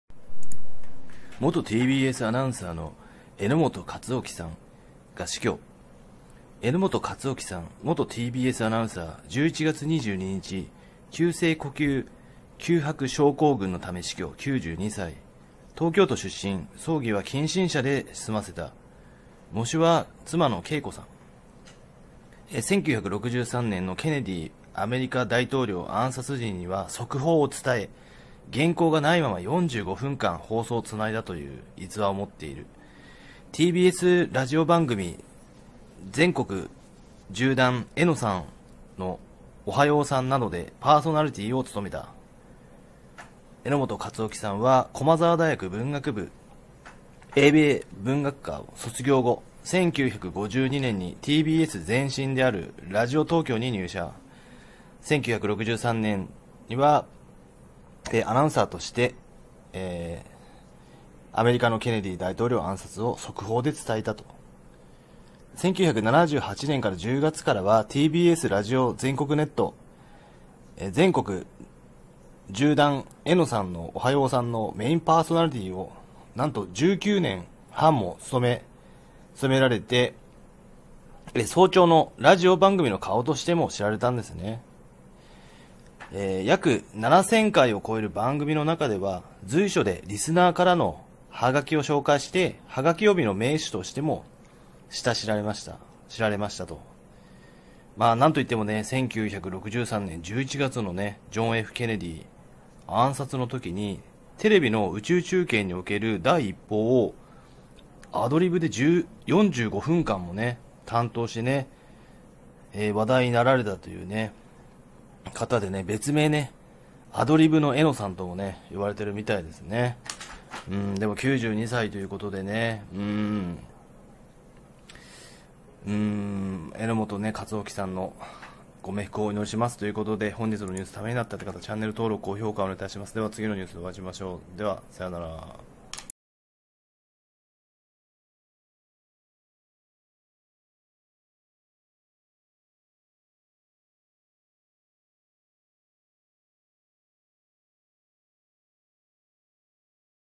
元TBSアナウンサーの榎本勝起さん死去
②関連動画はこちらです。
元ニッポン放送アナ・深澤弘さん死去、８５歳　ショウアップナイター名実況　親友のミスター「本当にショック」

榎本　勝起さん（えのもと・かつおき＝元TBSアナウンサー）11月22日、急性呼吸窮迫症候群のため死去、92歳。
　東京都出身。葬儀は近親者で済ませた。喪主は妻恵子（けいこ）さん。
　1963年のケネディ米大統領暗殺時には速報を伝え、原稿がないまま45分間、放送をつないだ。TBSラジオ番組「全国縦断・榎さんのおはようさん～！」などでパーソナリティーを務めた。

榎本勝起さんは駒澤大学文学部英米文学科卒業　 １９５２年、ＴＢＳの前身であるラジオ東京に入社。１９６３年には、アナウンサーとしてアメリカのケネディ大統領暗殺を速報で伝えました。

　１９７８年１０月からはＴＢＳラジオの全国ネット番組『全国縦断・榎さんのおはようさん～！』のメインパーソナリティーを１９年半にわたってつとめ、早朝のラジオ番組の顔として知られていました。